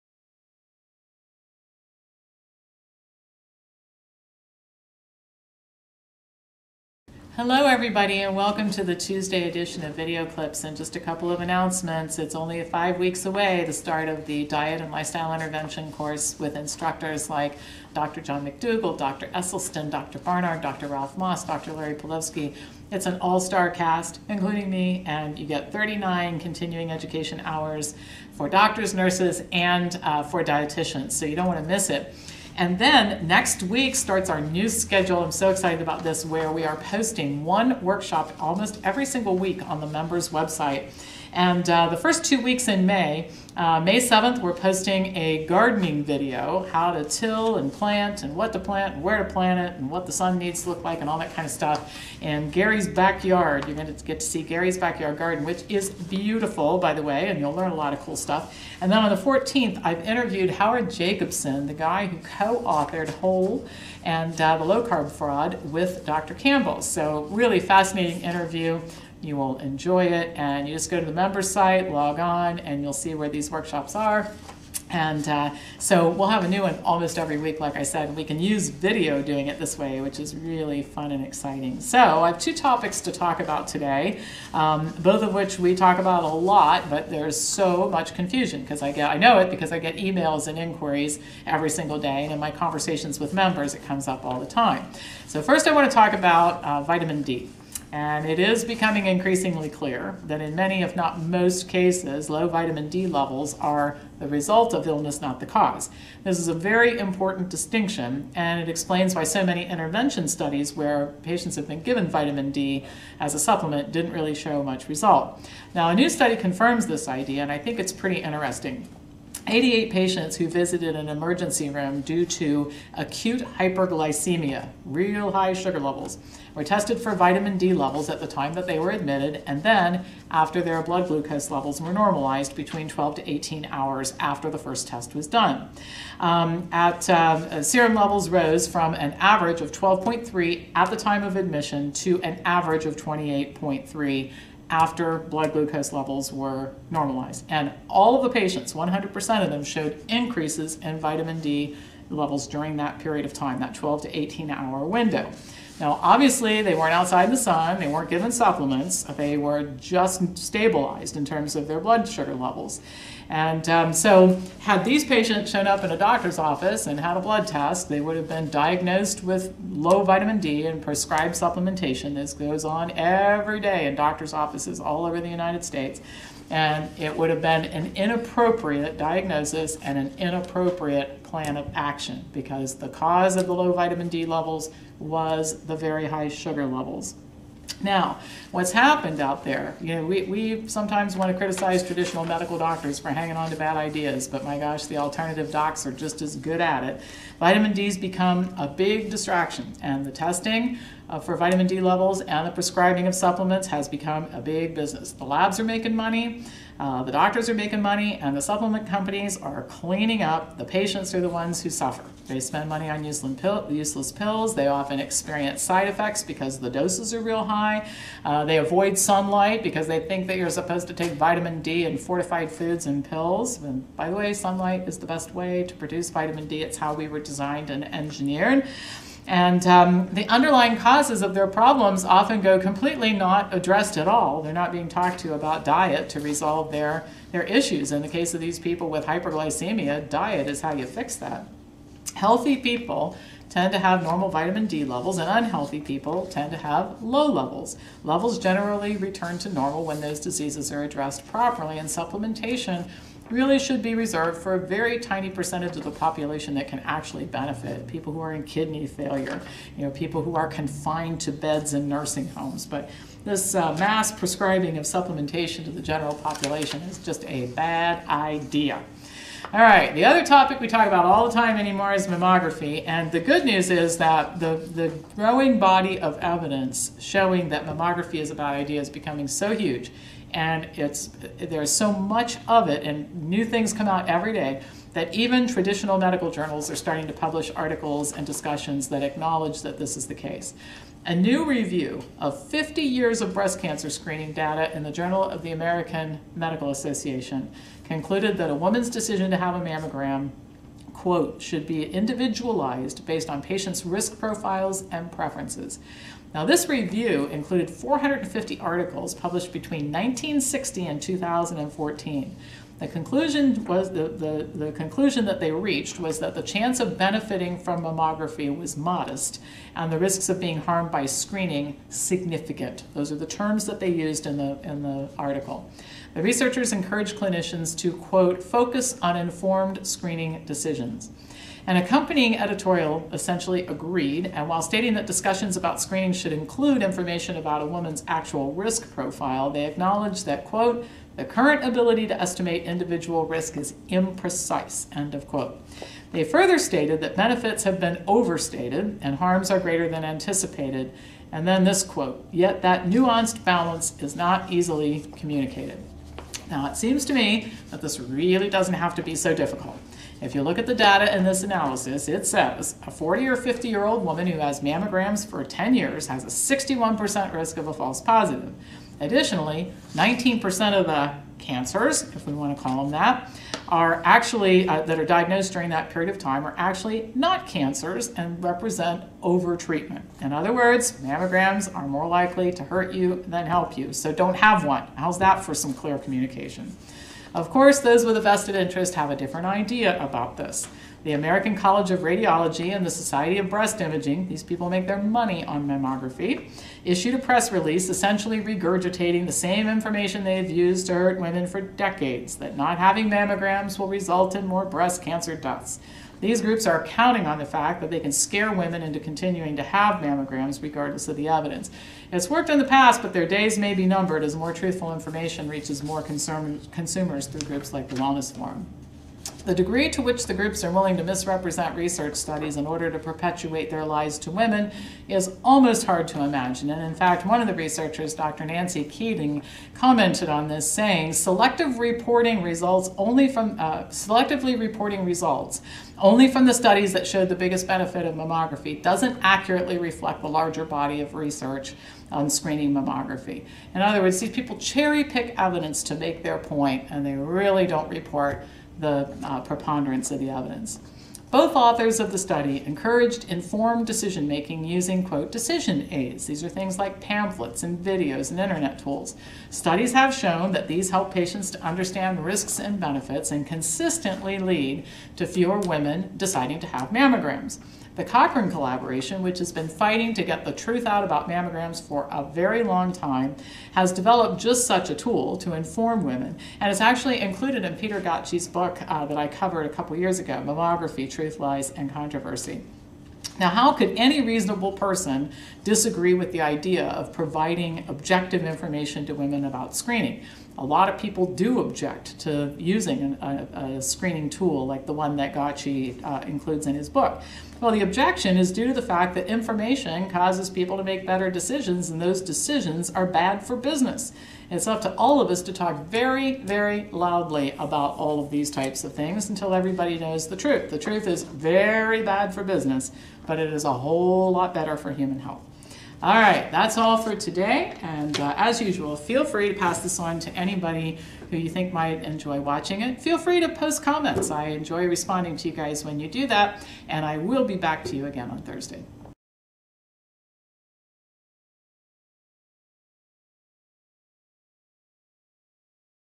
Dr Pam Popper: Vitamin D & Disease; Mammography: Risky Business
Low vitamin D levels are not the cause of disease, but rather a result of it.  A new study adds to the body of evidence that says that all of the testing and supplementing is not helping.  It's spring -- get out in the sun!!

And, the evidence is overwhelming; mammograms hurt more women than they help.  This latest analysis of hundreds of studies is hard to ignore.